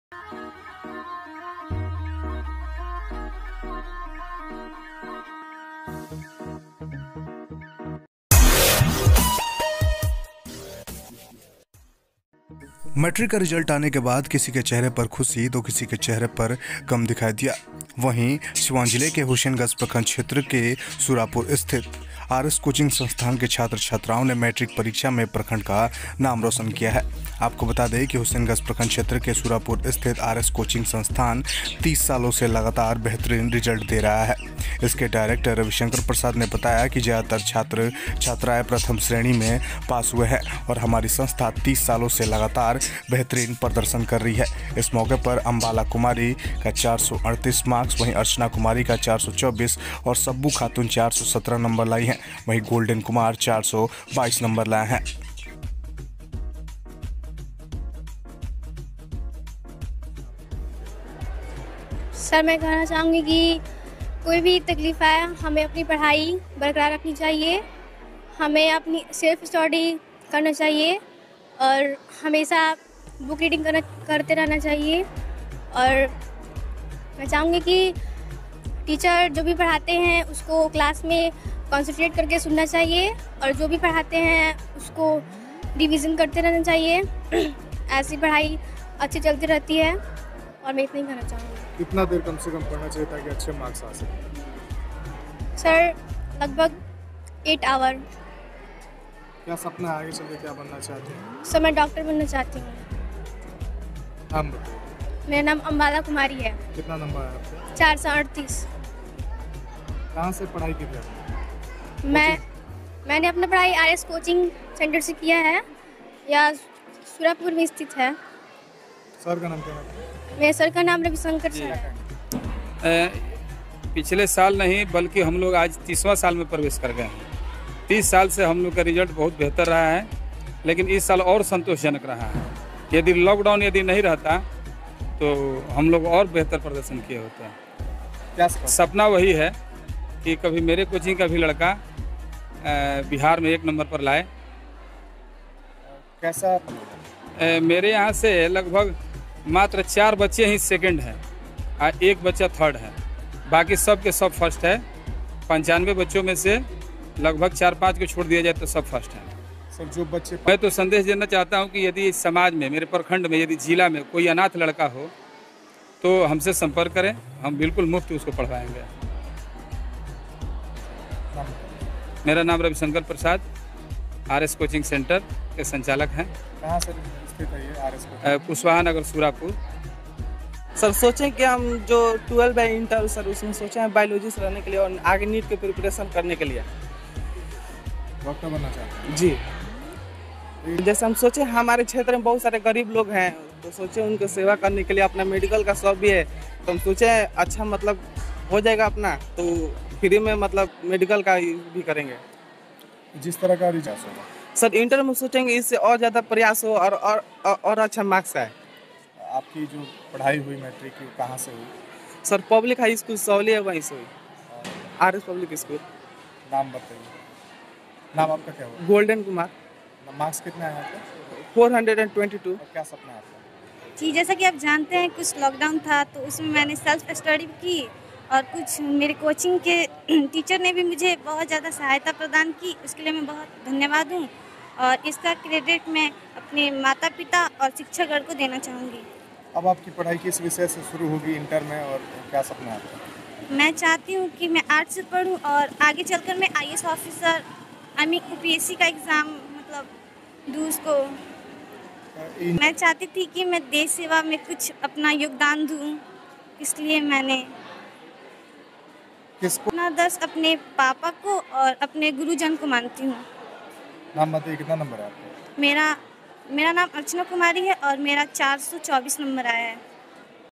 30 सालो से लगातार यह संस्थान दे रहा बेहतरीन रिजल्ट,छात्राओ ने किया कमाल....
bseb result
matric result news
Matric toppers
bihar board
siwa online
Siwan matric toppers
bihar matric toppers